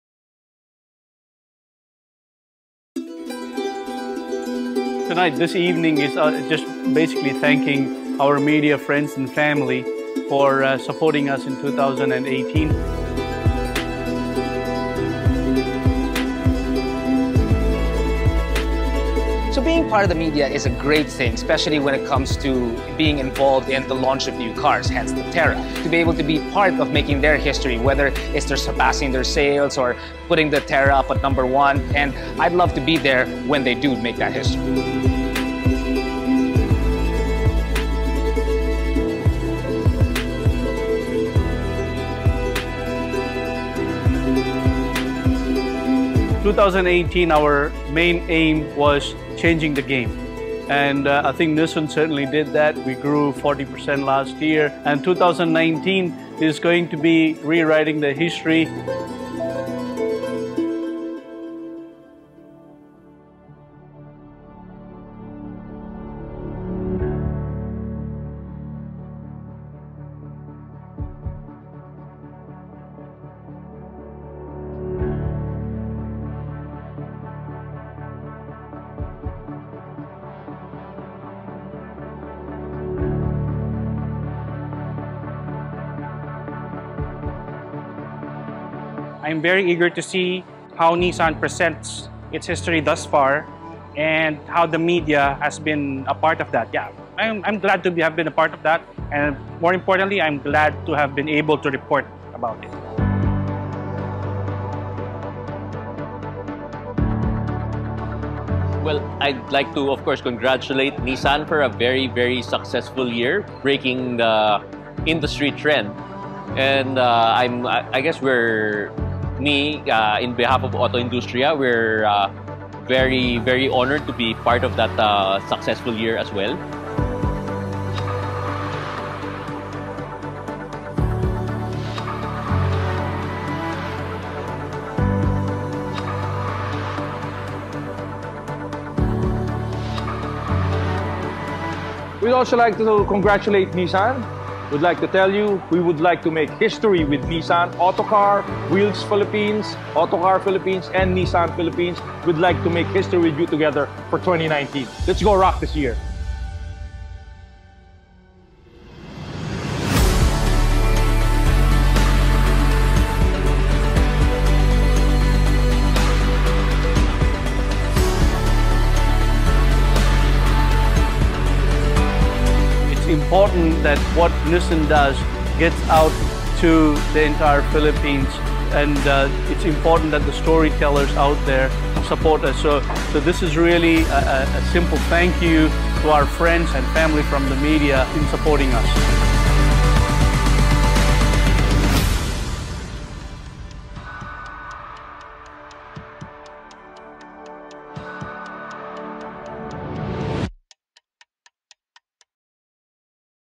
Making History - Media Thanksgiving 2019
After a game-changing 2018, Nissan Philippines pays tribute to members of the media for being part in making history with the company. Here are the highlights from that night.